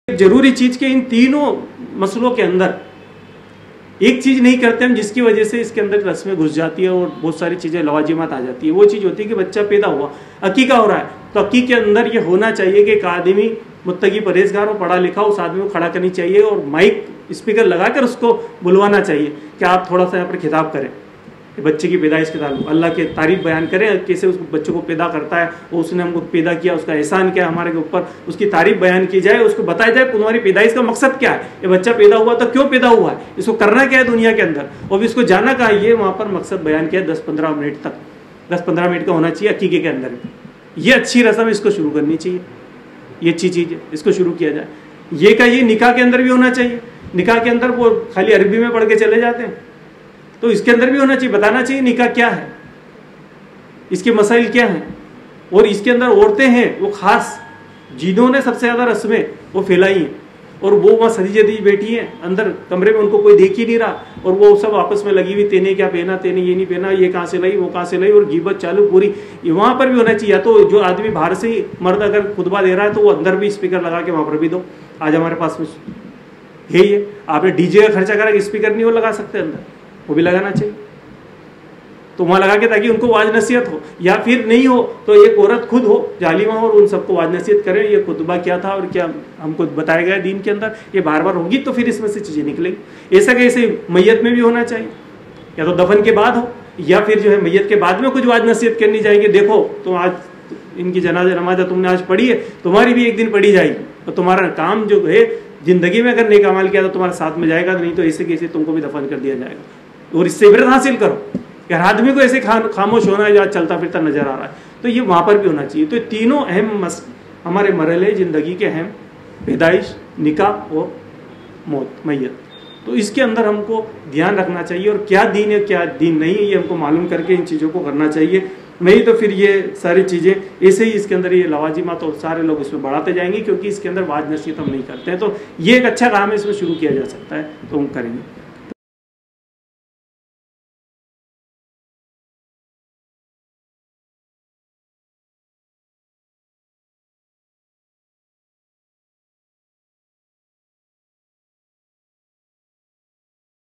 Akika Nikah Aur Mayyat Men Nasihat Hona Chahiye (अक़ीक़ा निकाह और मय्यत में नसीहत होना चाहिए)
अक़ीक़ा निकाह और मय्यत में नसीहत होना चाहिए
Speaker : Mujaffar Husain Mansuri
(Founder & President)

WEEKLY LECTURES
REFERENCE BOOKS LIBRARY
LITERATURE DISTRIBUTION
MARRIAGE BUREAU
ISLAMIC BANKING SYTEM
DINI EDUCATION

JAZAK ALLAH